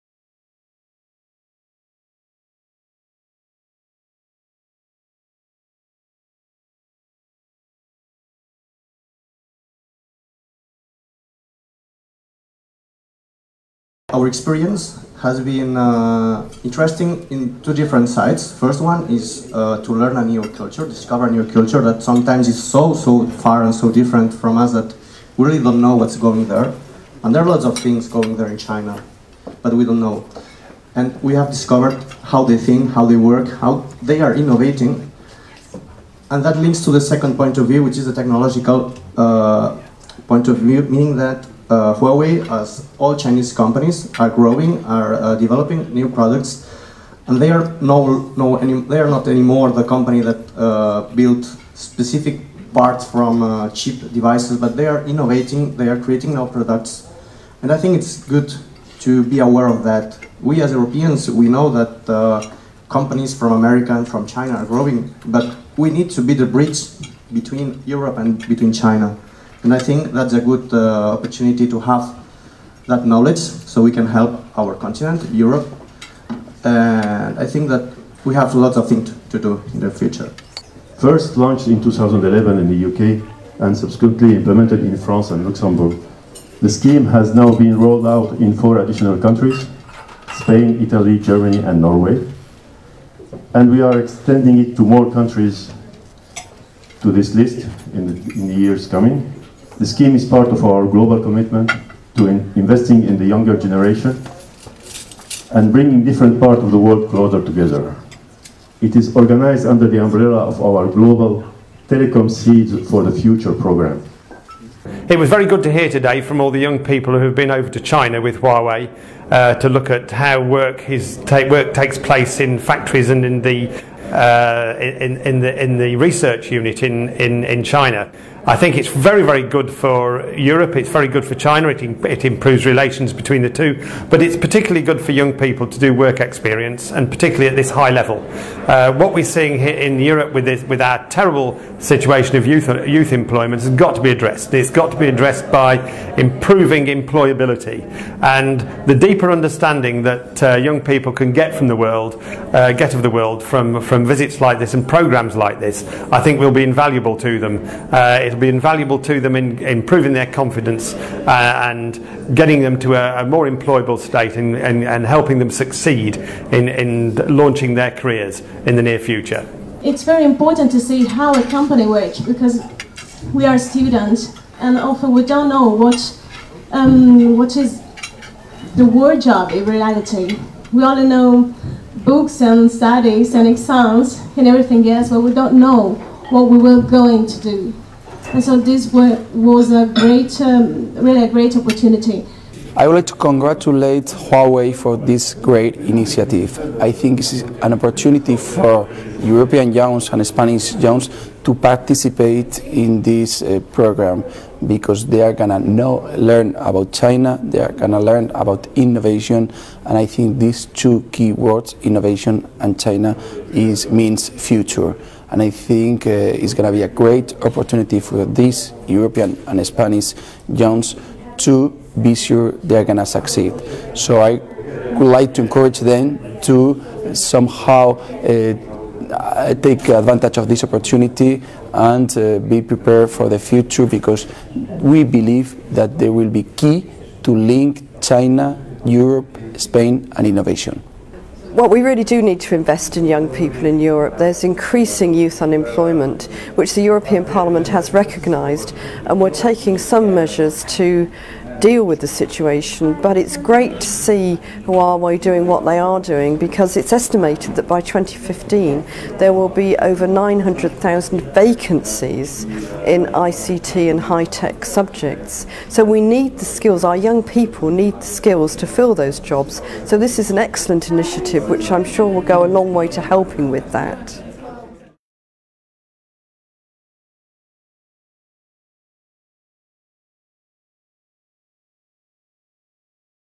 Creating Opportunities through Education: Huawei's Undergraduate Work Experience Scheme
Video produced by Kayleigh Lewis for Huawei and The Parliament Magazine.